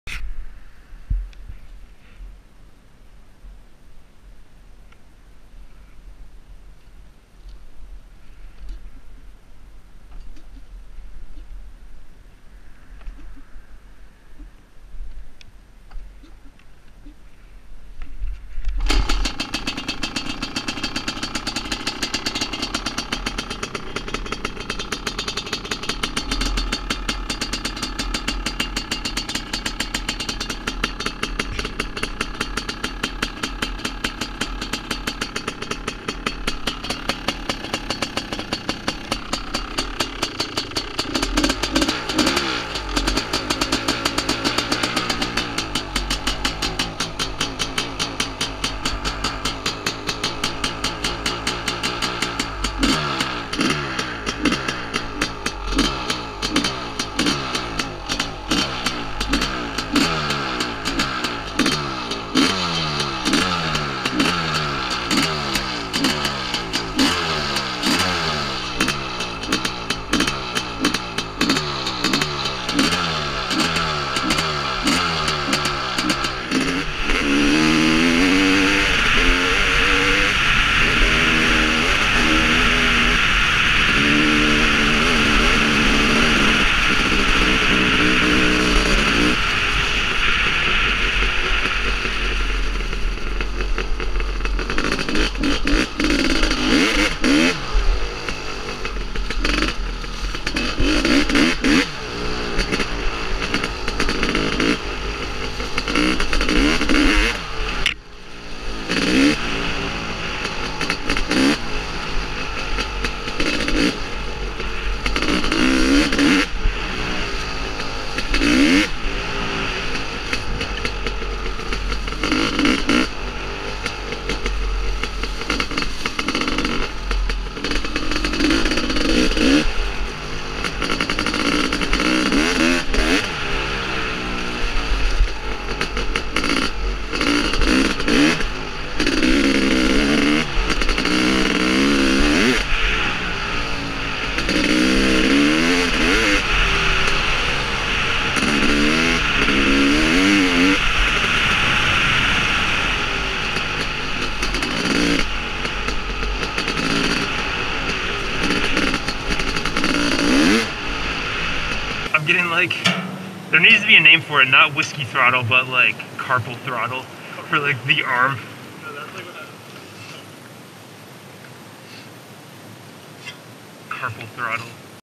CR500 lives?!?!
After a solid year of no starting, HERE IS THE BEAST.  Needs a top end for sure, but still a terrifying bike, especially since the rear brake is currently the spongiest thing on earth